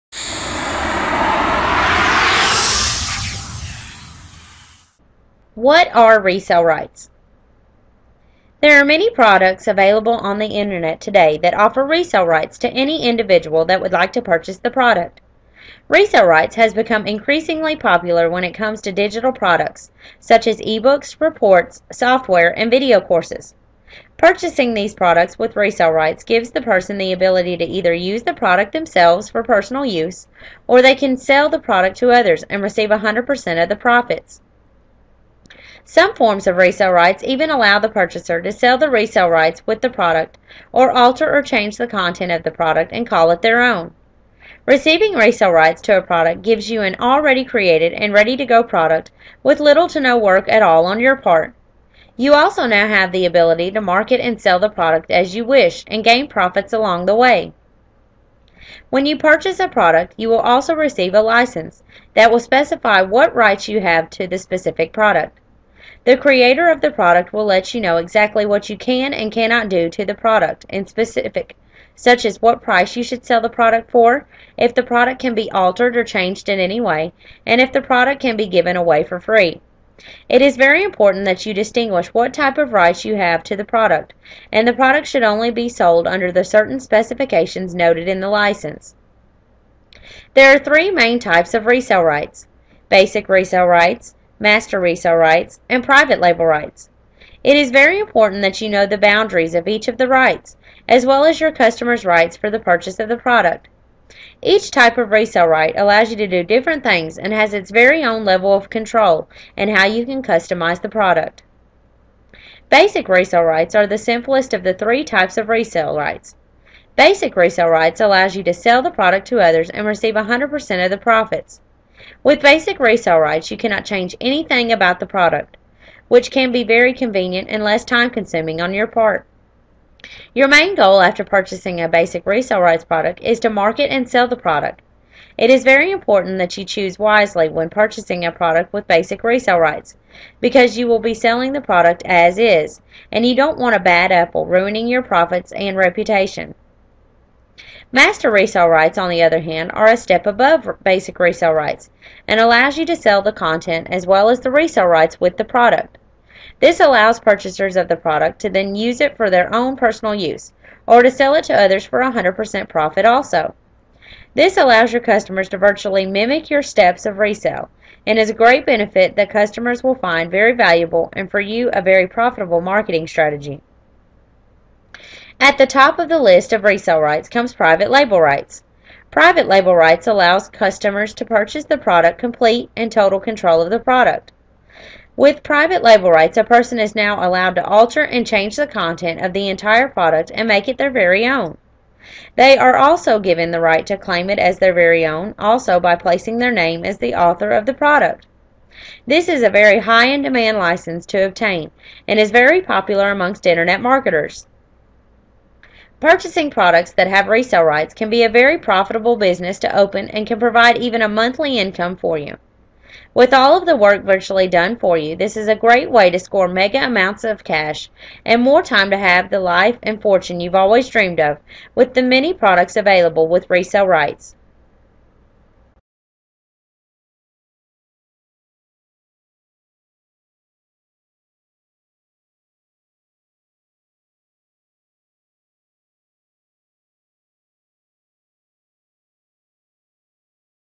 What Are Resell Rights
There are many products available on the internet today that offer resell rights to any individual that would like to purchase the product. Resell rights has become increasingly popular when it comes to digital products such as ebooks, reports, software, and video courses.